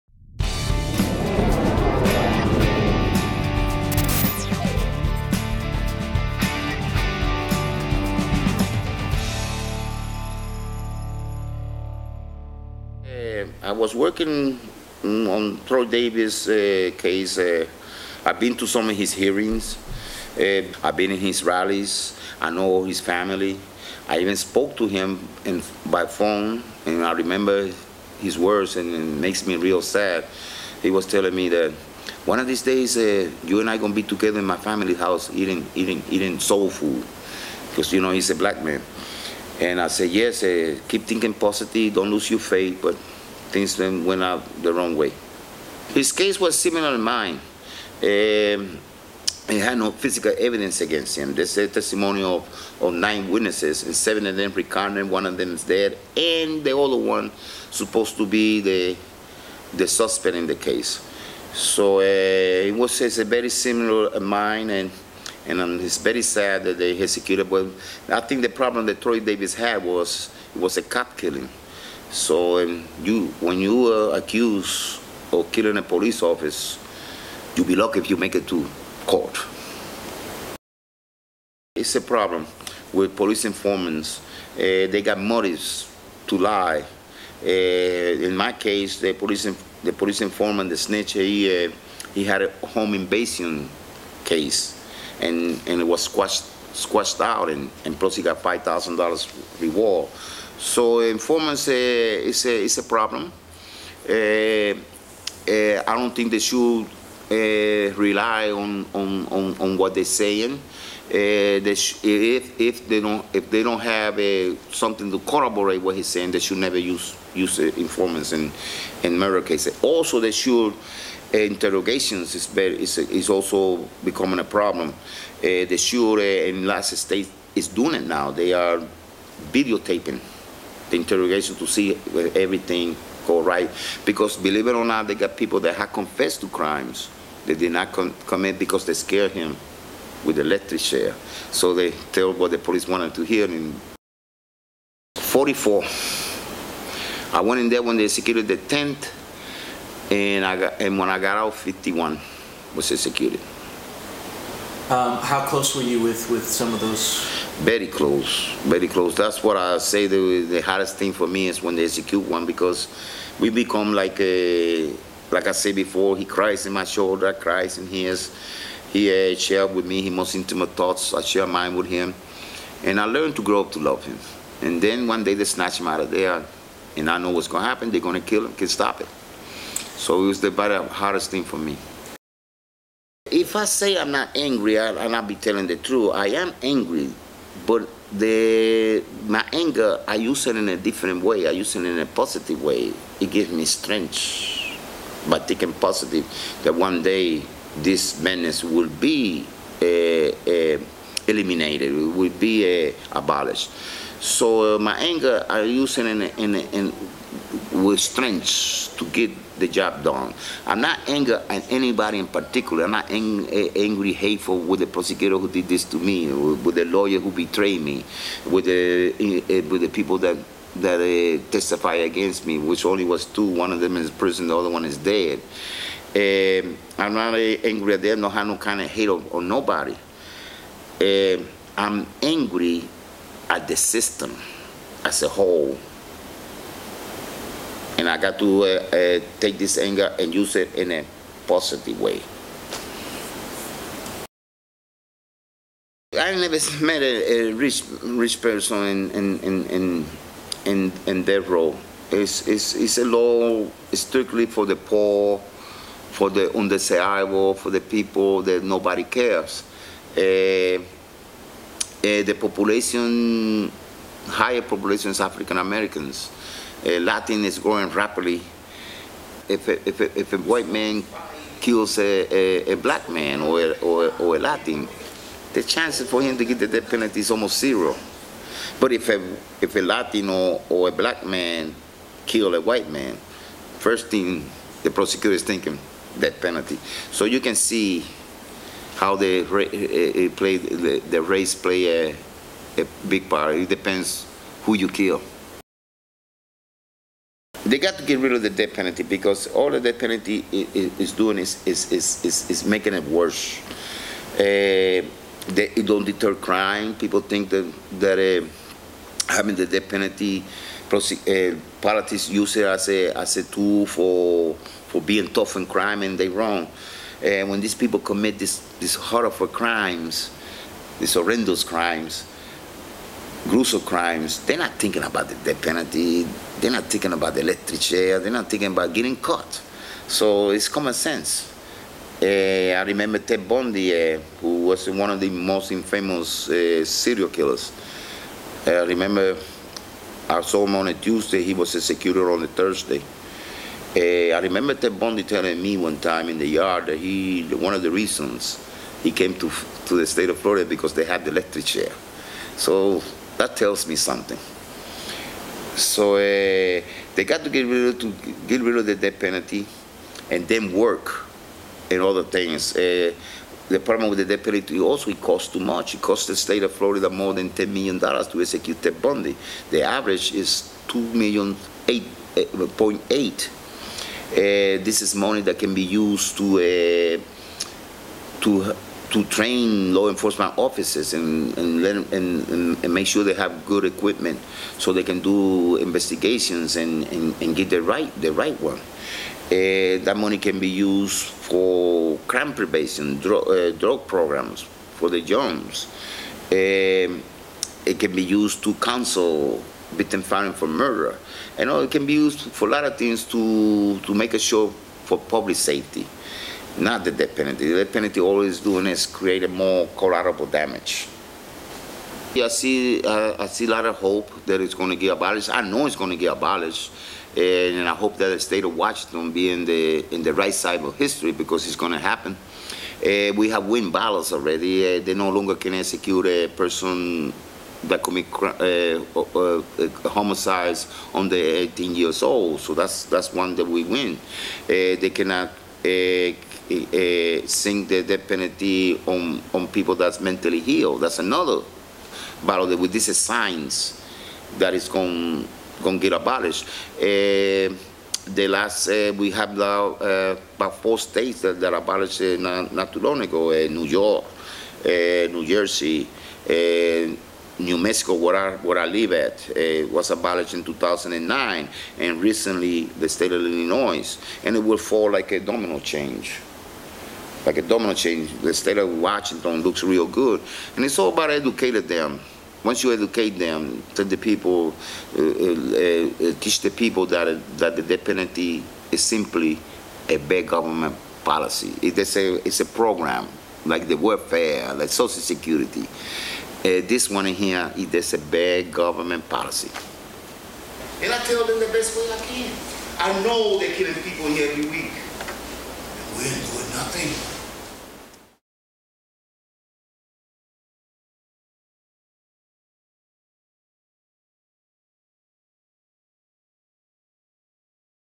Former death row inmate Juan Melendez visit Walla Walla
Death-penalty opponent and former death row inmate Juan Melendez spent a few days in Walla Walla speaking to students and the public at Whitman College and Walla Walla Community College.